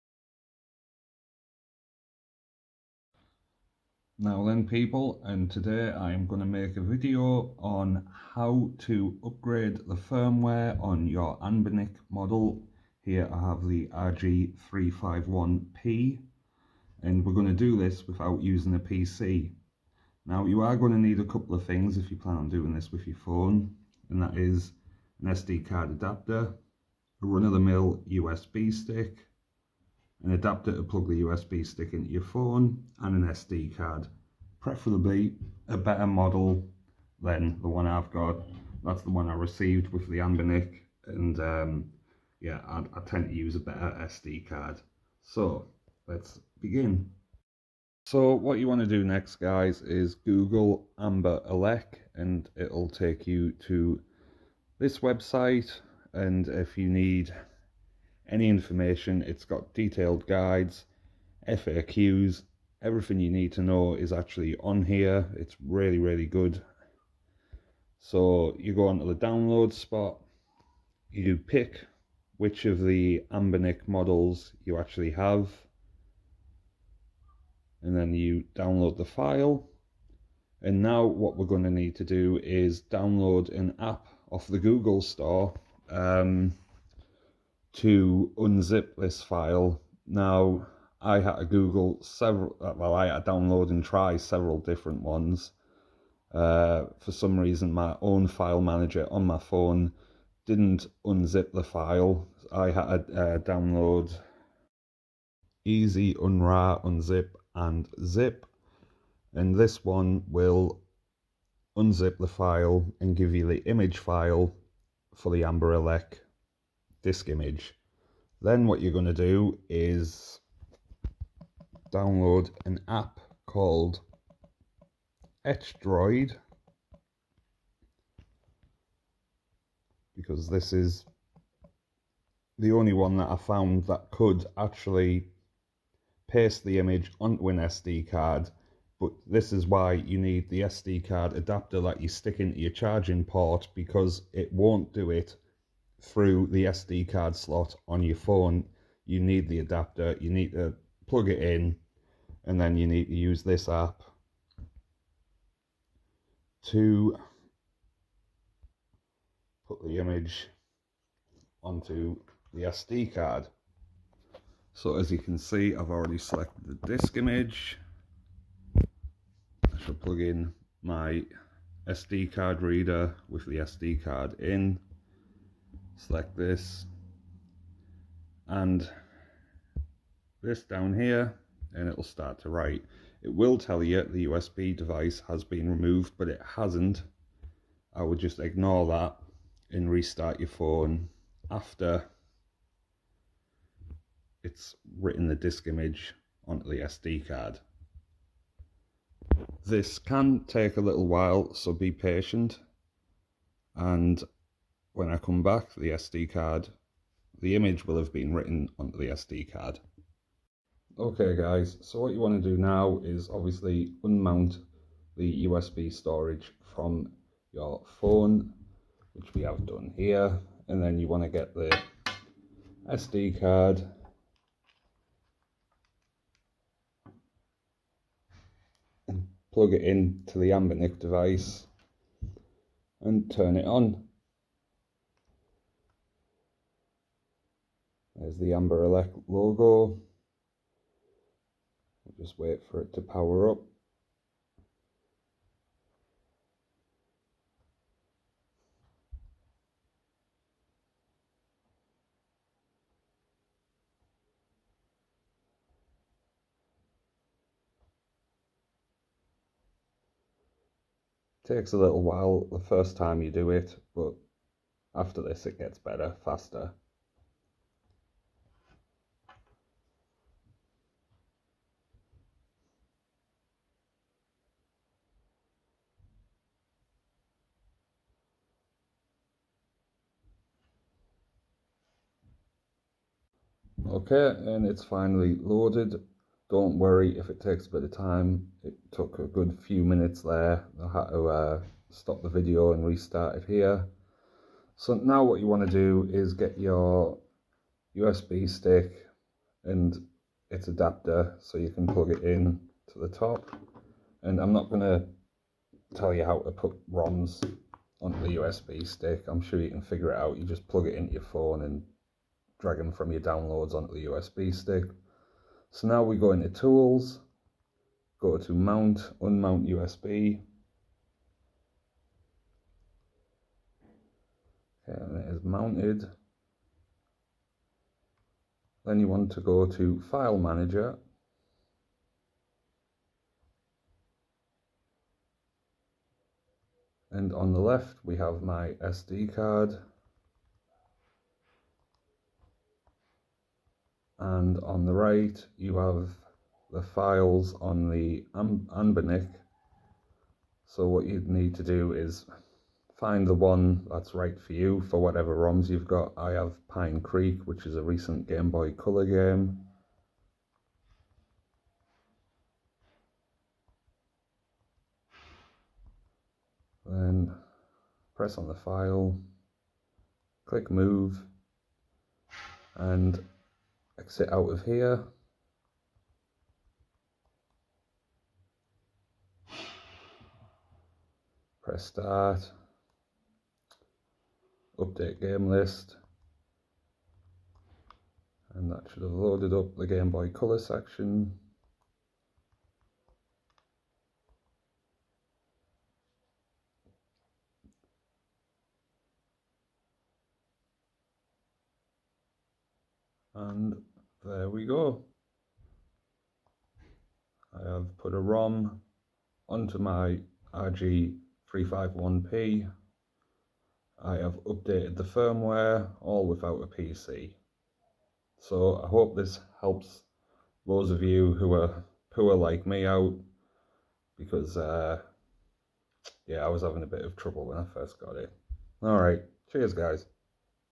Anbernic RG351P Update Firmware and add roms. No PC needed.
This is a guide for people who don't own a PC sp they can update there Anbernic Devices and add there own roms. For us poor people out there.